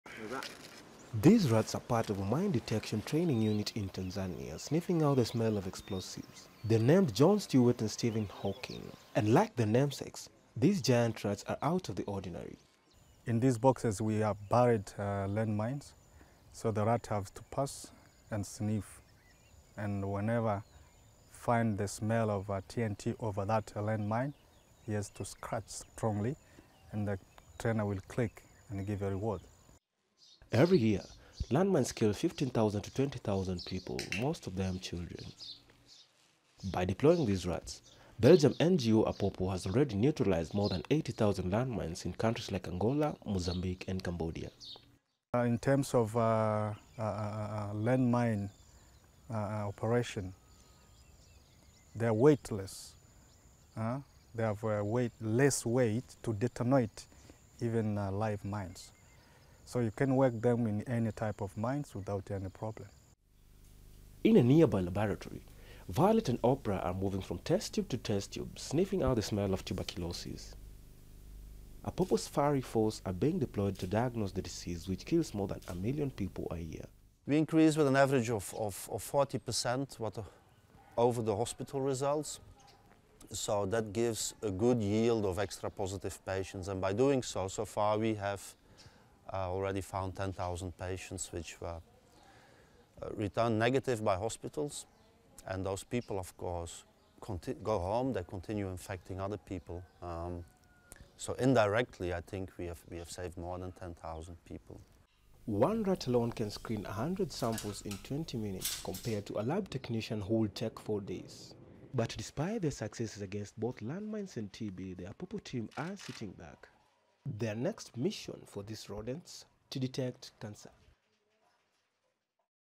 Rats with nose for trouble train to save lives in Tanzania
Rats are being trained to sniff out landmines and tuberculosis in Tanzania as part of a project which could one day use rodents to diagnose cancer. Landmines continue to maim and kill across Africa and Asia but with the rats' help the Belgian nonprofit organisation Apopo has already neutralised 83,000 of the weapons.